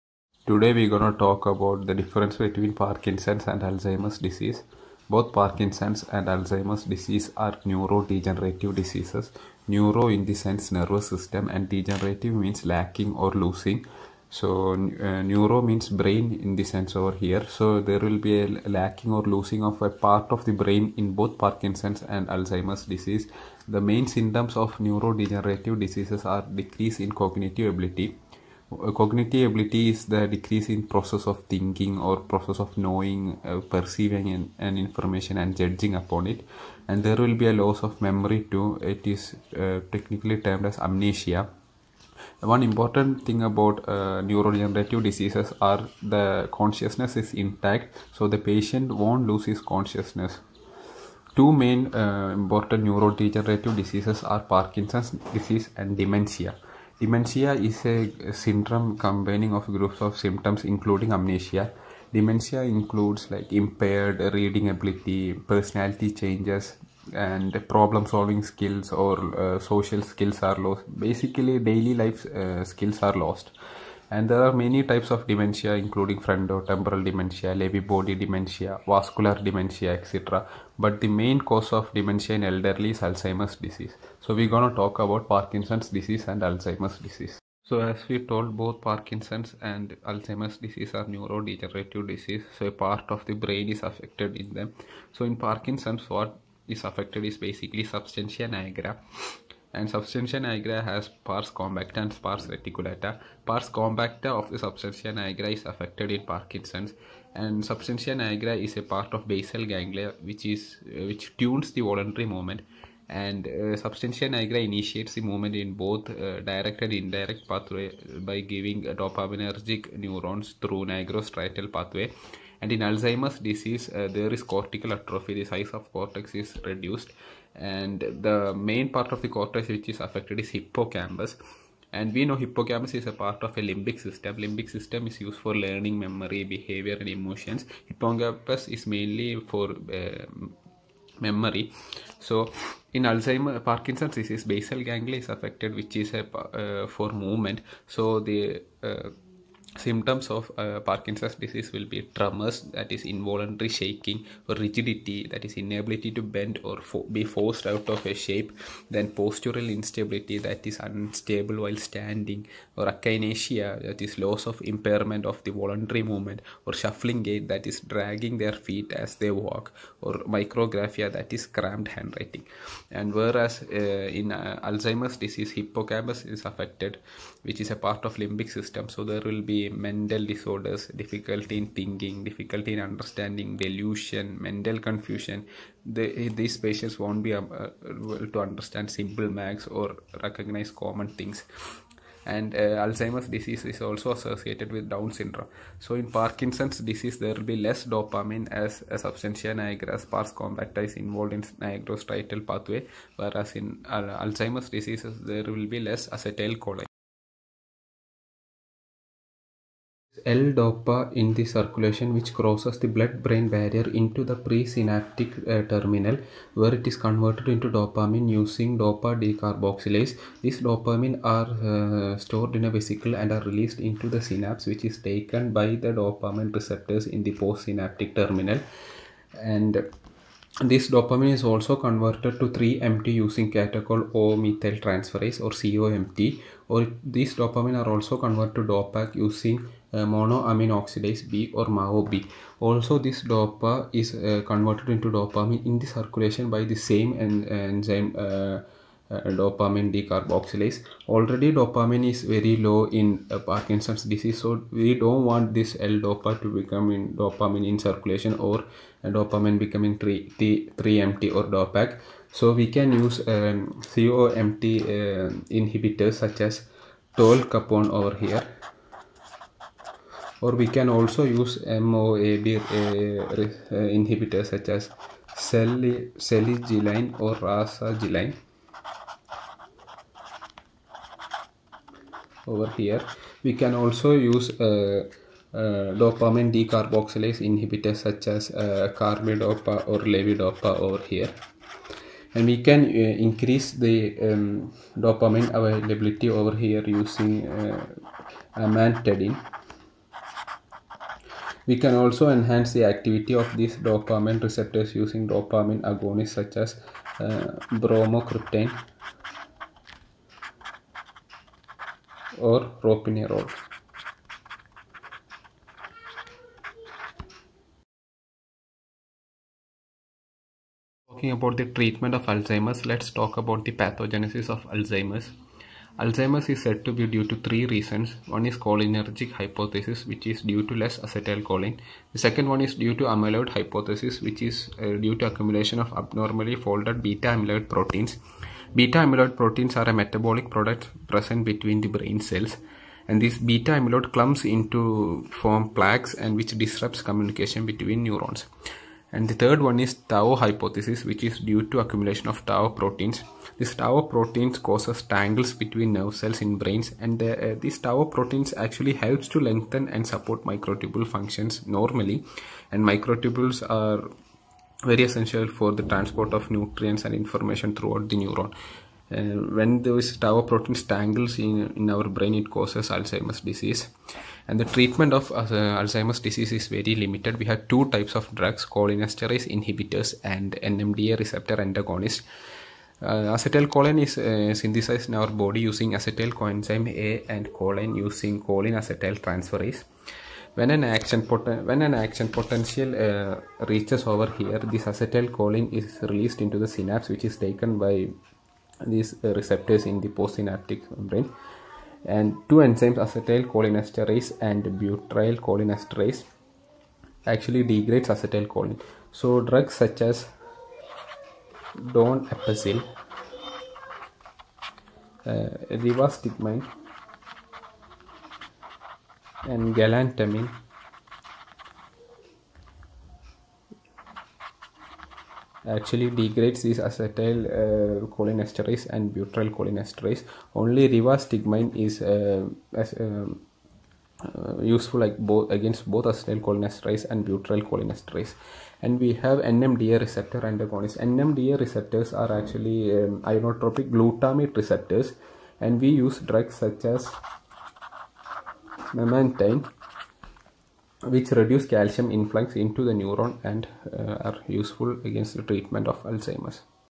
ALZHEIMER'S VS PARKINSONS(neurodegenerative disorders; overview, symptoms , pathology and treatment,
Alzheimer's and Parkinson's disease
A neurodegenerative disease
It's overview
Pathology
Symptoms
Treatment